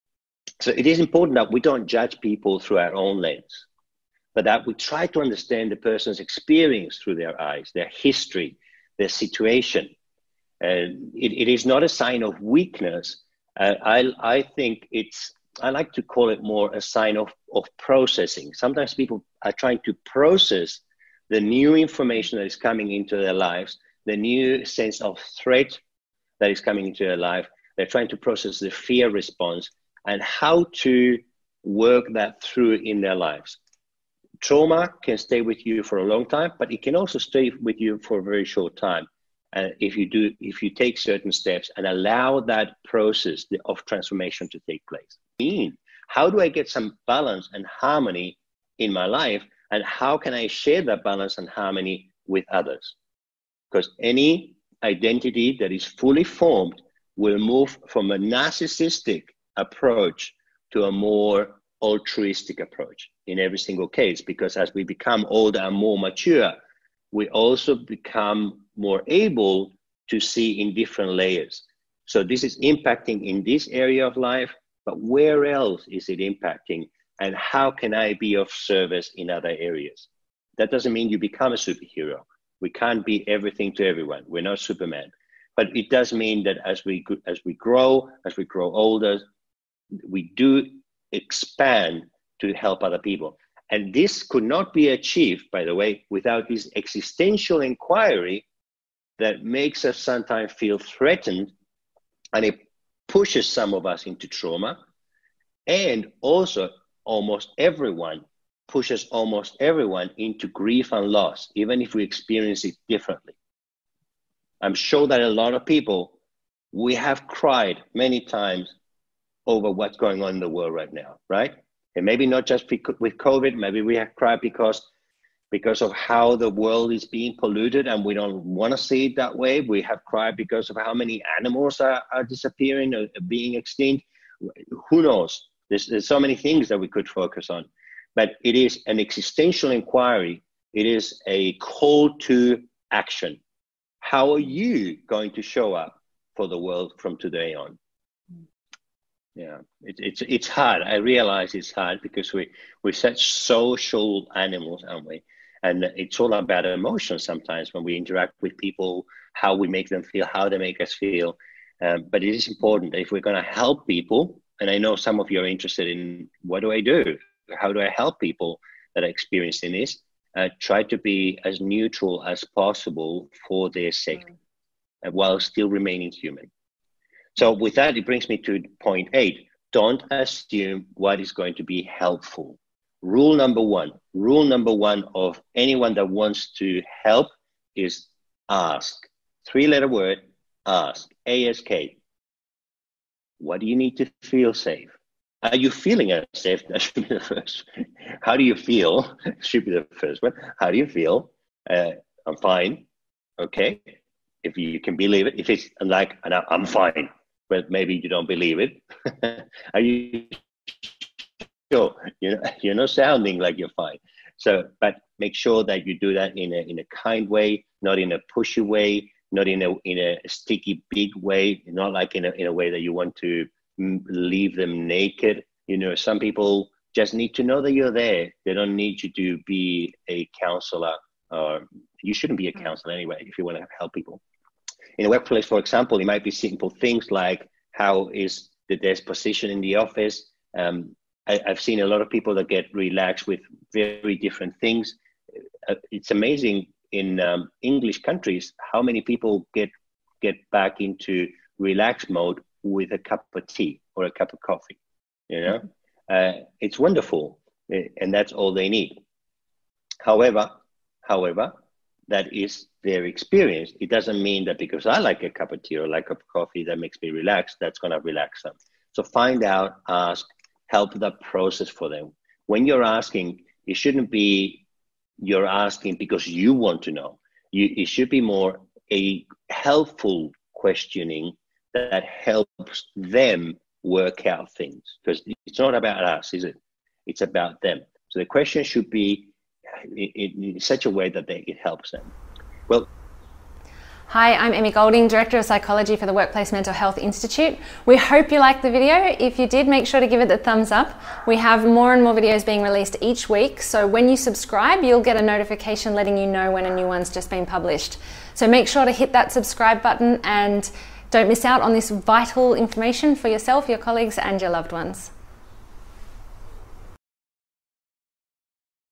How do we deal with someone experiencing trauma, emotional or psychological stress?
Trauma. All of us have likely experienced some form of emotional stress and anxiety, perhaps through personal grief or loss. Perhaps we feel depressed with what is happening in today’s world – COVID 19, racism, injustice, environmental abuse, war, and politics. How do we deal with trauma? How do we deal with someone experiencing trauma? Should we help them? HOW do we help them? How do we approach them?  What questions should we ask? In this video, Peter Diaz, mental health authority and CEO of the Workplace Mental Health Institute, offers expert advice about trauma and what we can do to help someone experiencing trauma.

This video is a clip from the WMHI Facebook Mental health LIVE discussing the topics of Trauma, PTSD (Post Traumatic Stress Disorder) Grief, and Loss with Peter Diaz and Emi Golding (WMHI Director of Psychology).